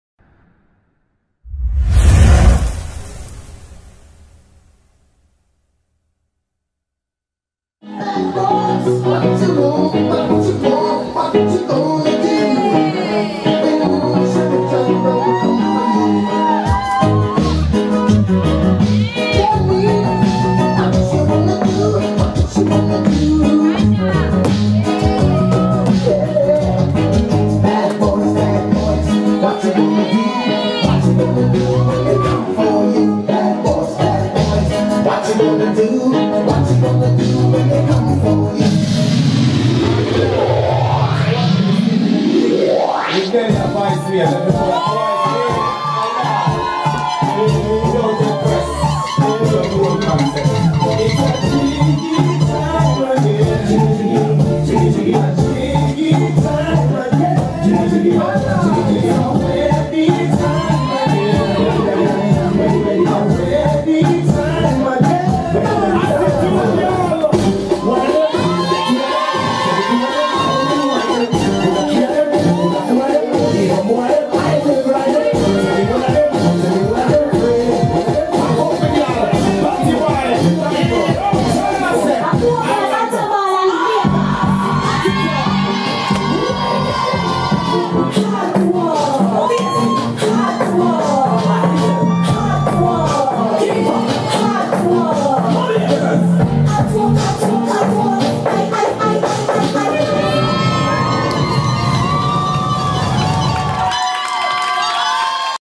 Katarzyna 'Mad Katie' Kuwałek / R1 / Slavic Dancehall Queen 2015
Występ Katarzyny 'Mad Kaie' Kuwałek na Slavic Dancehall Queen 2015 podczas pierwszej rundy.